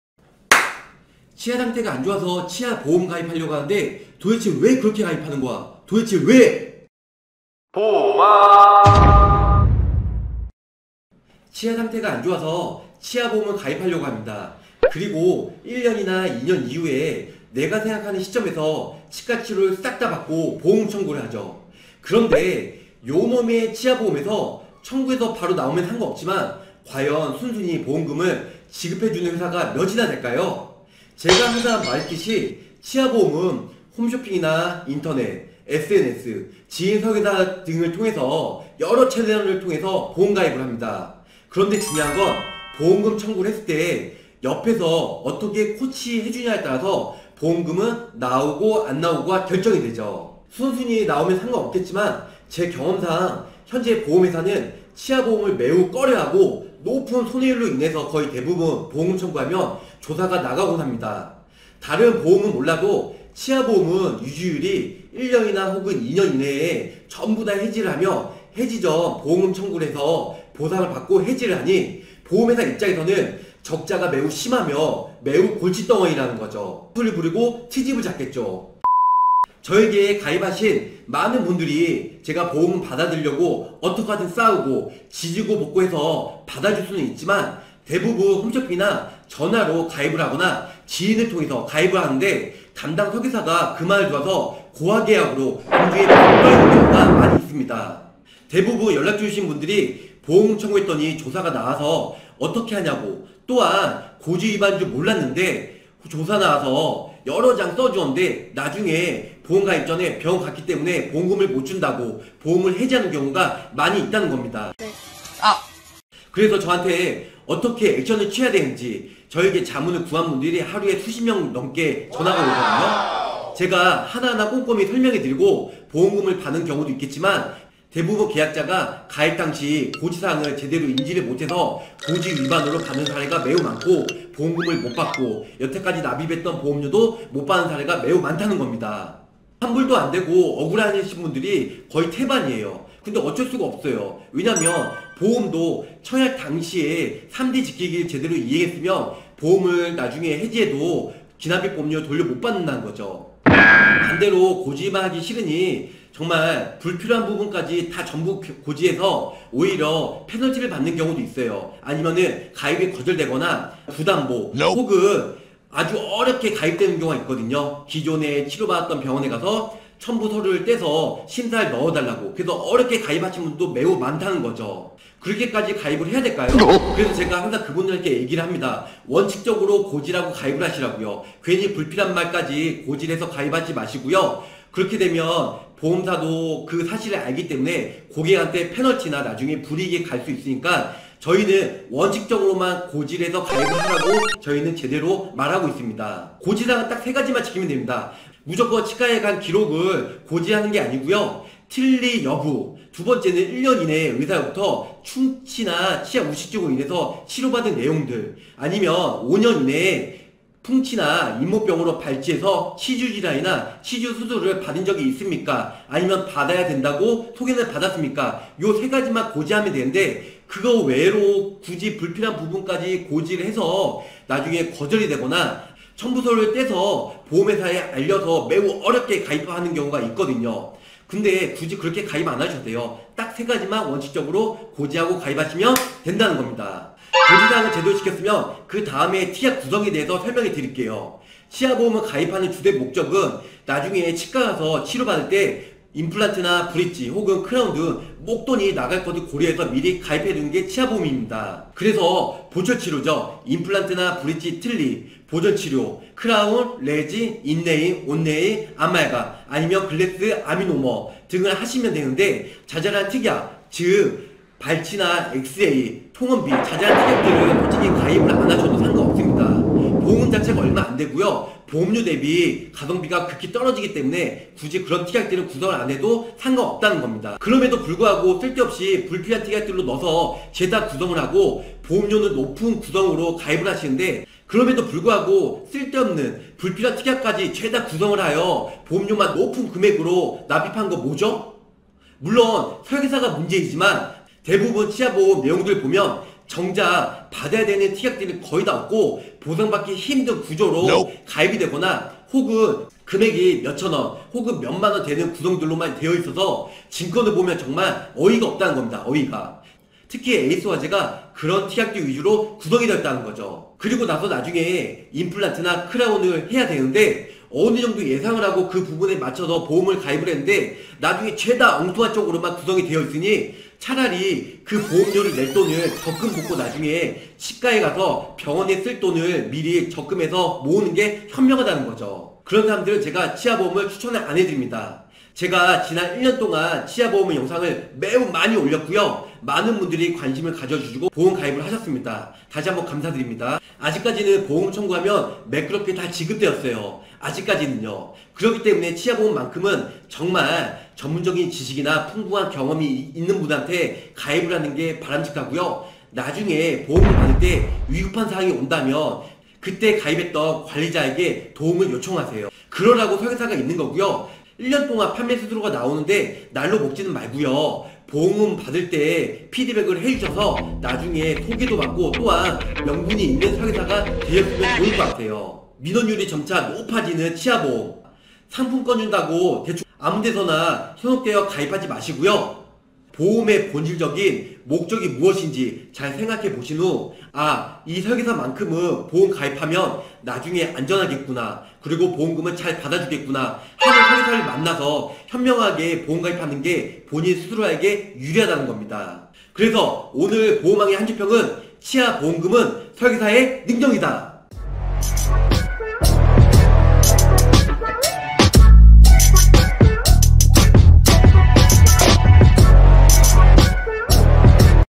치아보험 제발~좀 그렇게 가입하지마! (후회 100%)
안녕하세요 치아전문가 보험왕입니다.

지난 1년동안 치아보험 보험가입 문의.보상문의 청구등
치아보험의 대해서는 많은 노하우를 가지고 있습니다.

저에게 가입하신분외에 .. 정말 쓸데없는 담보와 보장가지고
하소연 하신분들을 위하여 영상을 준비해 보았습니다.

많은 도움되십시요^^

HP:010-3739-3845
카톡친구추가:r2an
카톡플러스친구:태수의 보험리모델링